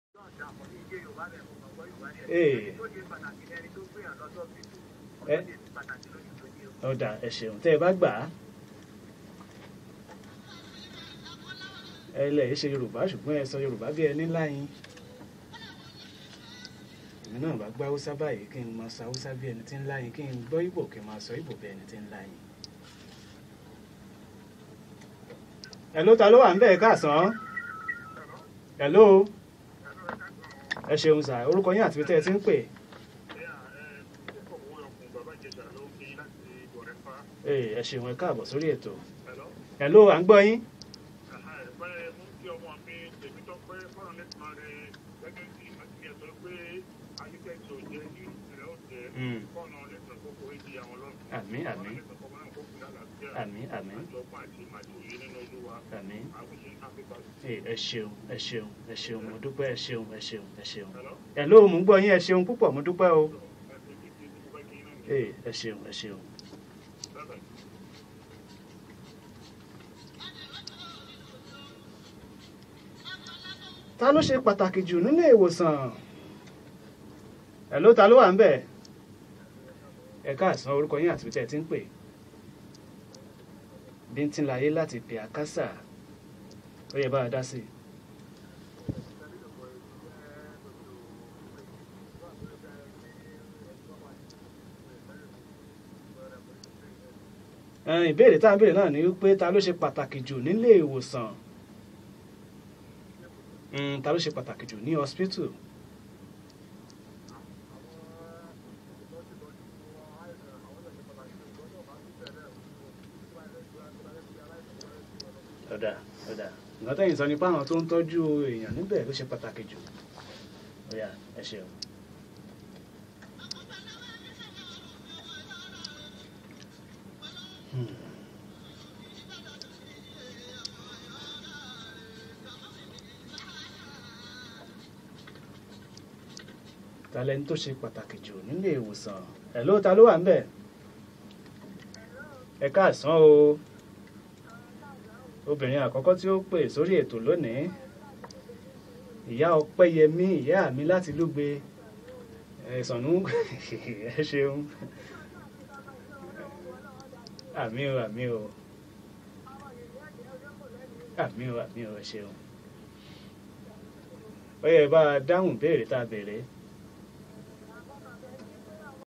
Who Is The Most Important Person In The Hospital?
The question, who is the most important in an hospital to some seems, controversial, while to some it is straightforward answer.
A lots of answers were given. You get to know the answer through this video.